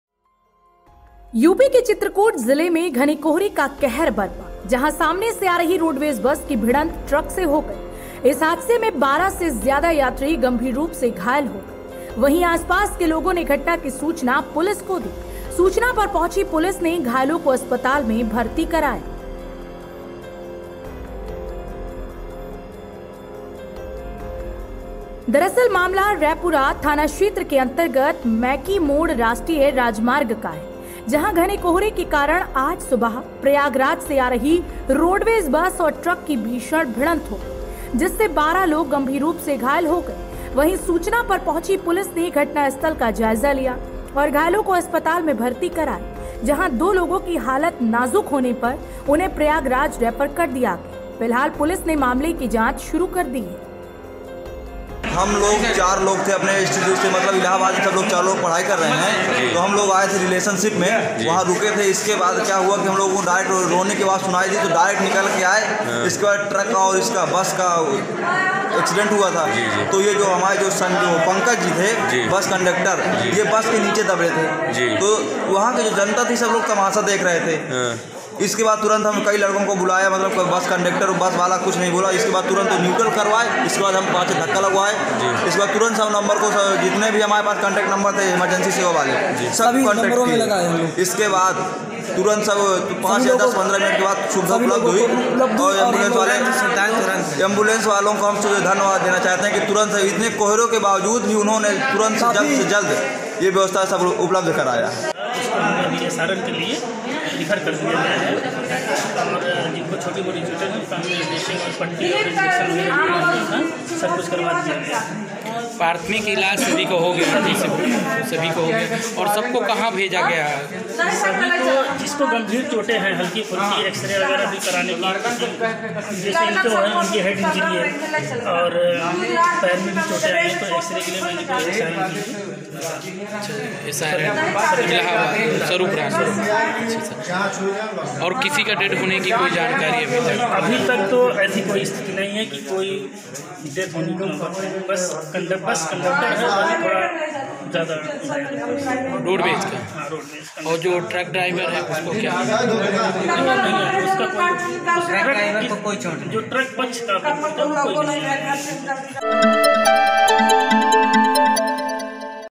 चित्रकूटः ट्रक और बस की आमने सामने हुई भिड़ंत 12 लोग हुए घायल
यूपी के चित्रकूट जिले में घने कोहरे का कहर बरपा...जहां सामने से आ रही रोडवेज बस की भिड़ंत ट्रक से हो गई..इस हादसे में 12 से ज्यादा यात्री गंभीर रुप से घायल हो गए..वही आस-पास के लोगों ने घटना की सूचना पुलिस को दी..सूचना पर पहुंची पुलिस ने घायलों को अस्पताल में भर्ती कराया। दरअसल मामला रैपुरा थाना क्षेत्र के अंतर्गत मैकी मोड़ राष्ट्रीय रजयमार्ग का हैं जहां घने कोहरे के कारण आज सुबह प्रयागराज से आ रही रोडवेज बस और ट्रक की भीषण भिड़ंत हो गई है जिससे 12 लोग गंभीर रुप से घायल हो गए हैं वहीं सूचना पर पहुंची पुलिस ने घटनास्थल का जायजा लिया और घायलों को अस्पताल में भर्ती कराया जहां 2 लोग की हालत नाजुक होने पर उन्हे प्रयागराज रिफर कर दिया गया। फिहाल पुलिस ने मामले की जांच शुरु कर दी हैं।